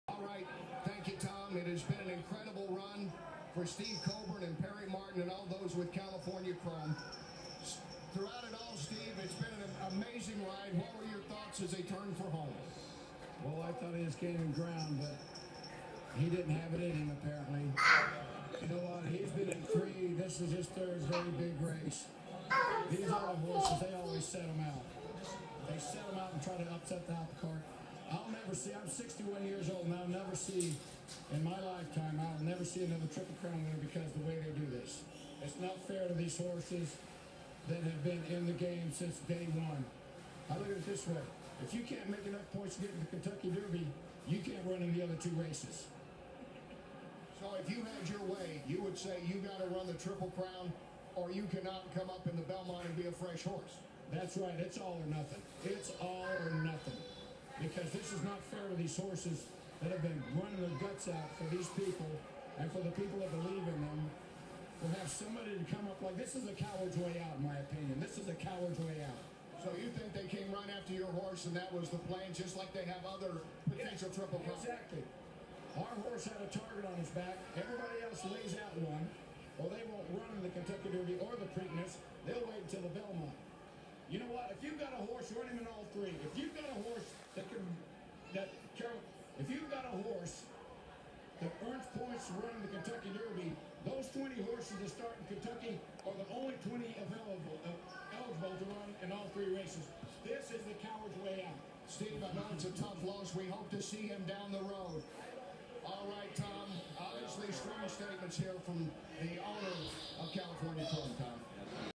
Steve Coburn and his wife - Reaction to California Chrome L
Steve Coburn and his wife - Reaction to California Chrome Loss